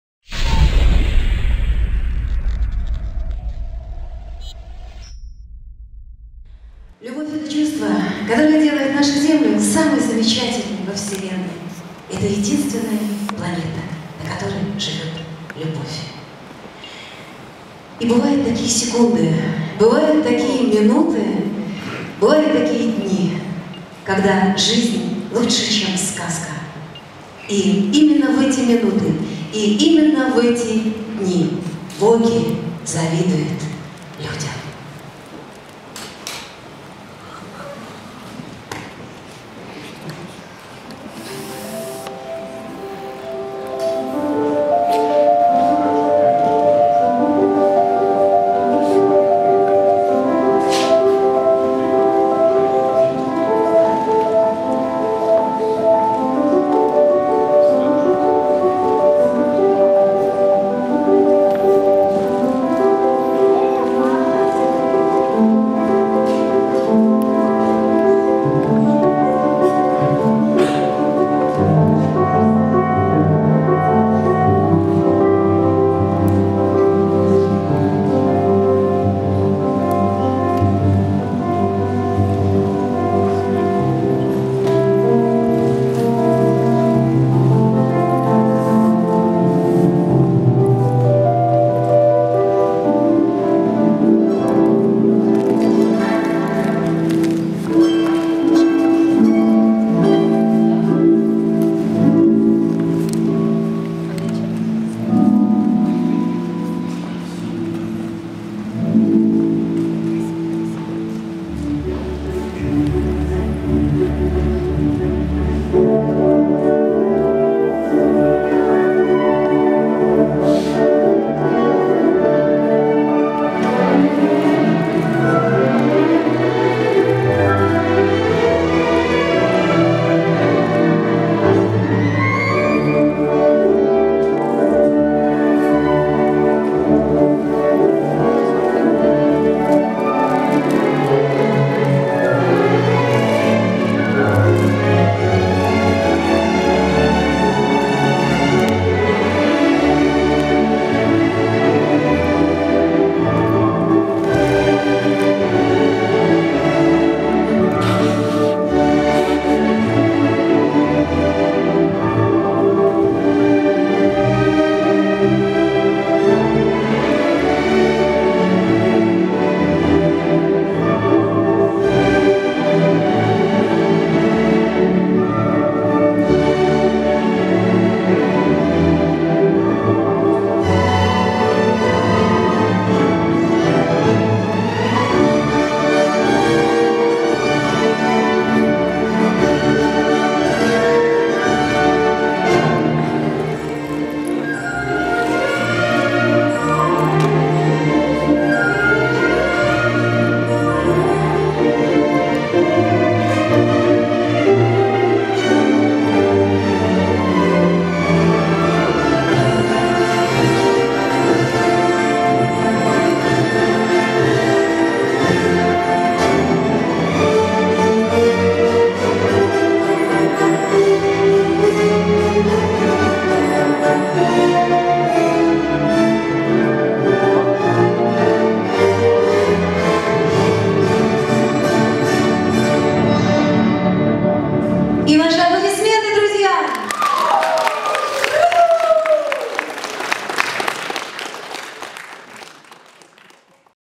Wedding projection show
Этот проект – наш первый опыт участия в церемонии бракосочетания!

Это радостное событие происходило в зале Харьковской Филармонии. Яркое проекционное представление под бессмертную музыку Баха и Чайковского наверняка запомнилось не только жениху и невесте, но и их гостям!